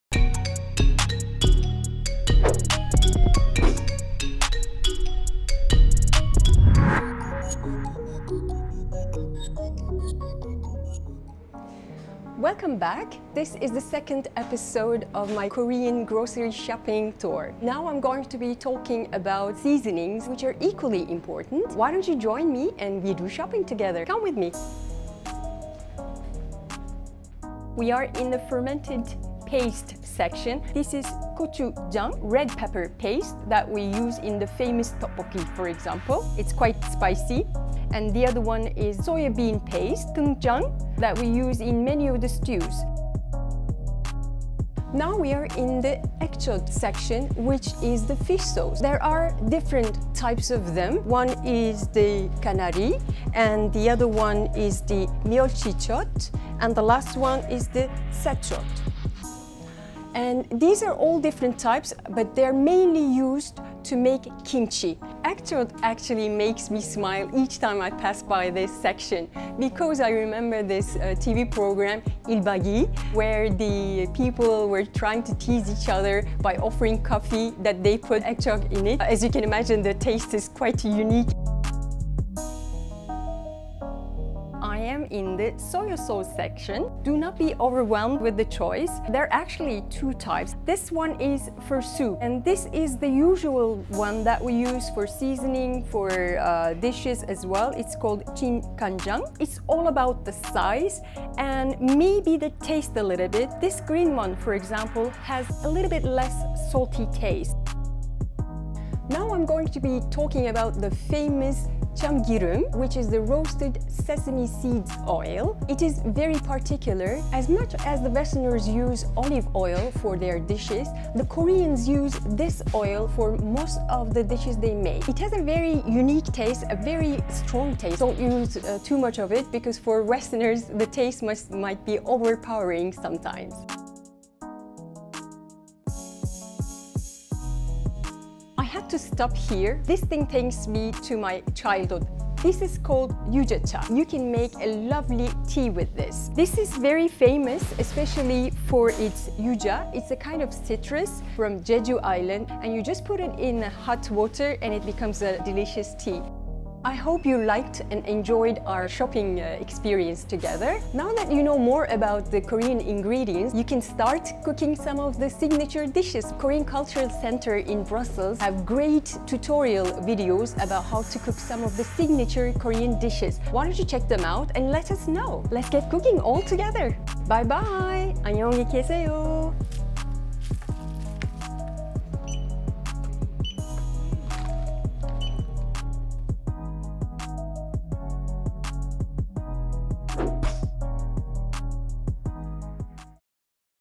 KCC - Korean Grocery Shopping
Second episode of a Korean grocery store tour, showcasing the most iconic ingredients of Korean cuisine. Commissioned by the Korean Cultural Center in Brussels.